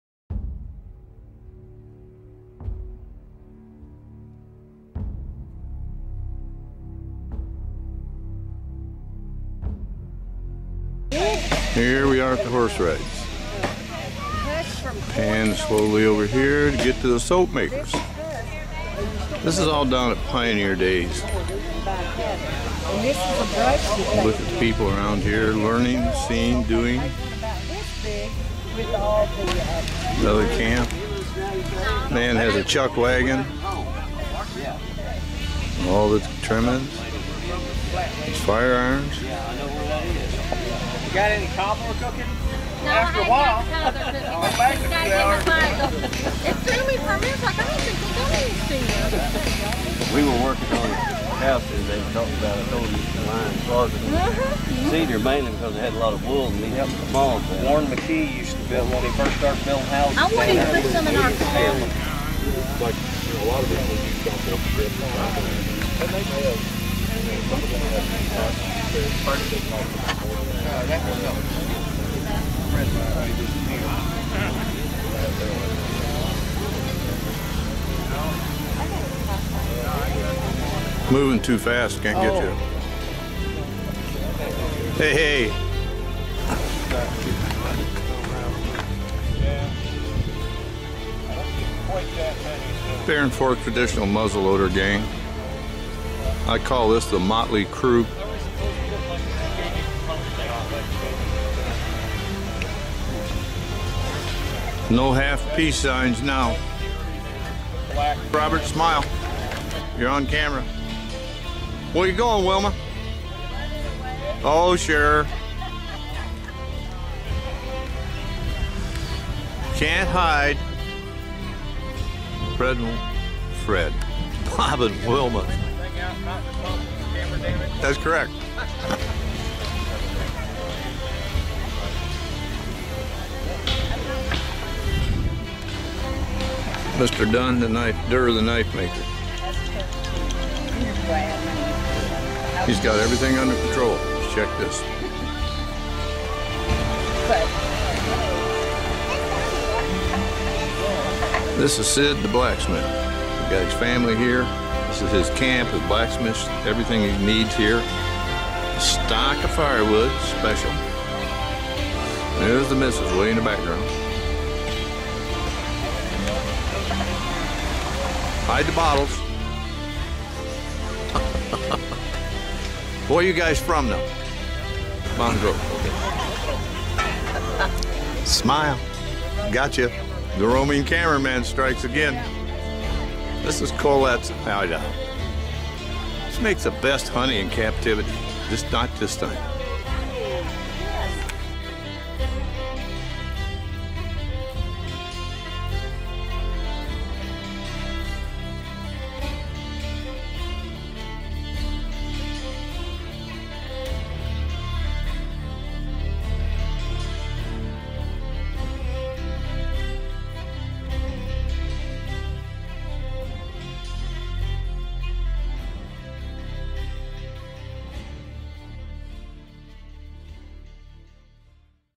2015 Pioneer Descendants Days - Southern Missouri, outside of Ava, USA
Rendezvous enthusiasts join members of the Barren Fork Traditional Muzzle Loaders Association, otherwise known as the Barren Fork Muzzle Loaders Club, deep in Southern Missouri river country for a weekend gathering in October 2015 to celebrate pre-1840 frontier America. Wagon rides, pioneer cooking, historical demonstrations on the methods for making brooms, soaps, knives, scrimshaw art, period clothing, etc. by hand. Authentic encampments with tents, teepees, and covered wagons. Muzzleloading competition. Period dress is encouraged, but not required. Interesting, educational, and fun way to enjoy a beautiful fall day/weekend for both rendezvous enthusiasts and the general public alike.